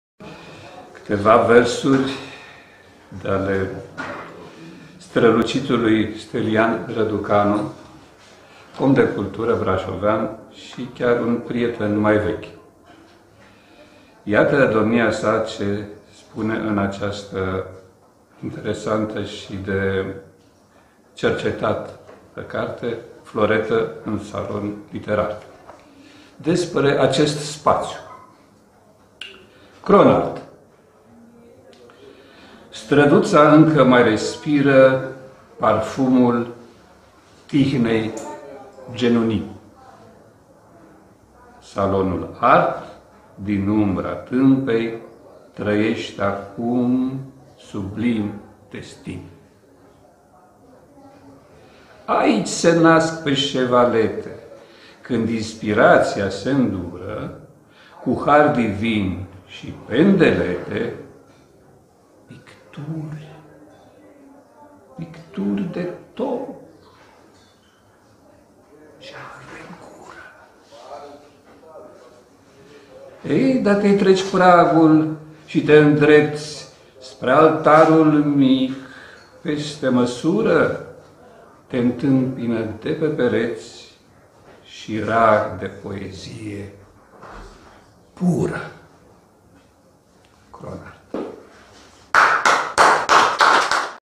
George Custură, moment poetic
George Custură, moment poetic. Lansare de carte: „Floretă în salon literar”, autor Stelian Răducanu, Editura Magic Print, Onești, 2021. Eveniment desfășurat la Galeria Kron Art, str. Postăvarului nr. 18, Brașov.